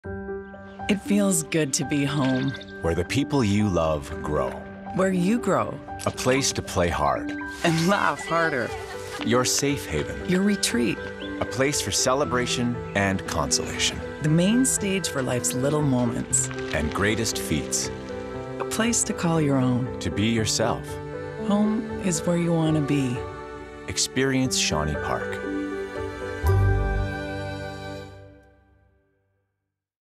Experience Shawnee Park
Shawnee Park is Calgary’s most exciting southwest community. From the curation of the last developable land bordering Fish Creek Park comes the start of a remarkable, master-planned community. Under the development leadership of Cardel Homes and Genstar Development Company, Shawnee Park features the best aspects of a new community while also encompassing the desirable aspects of a mature one.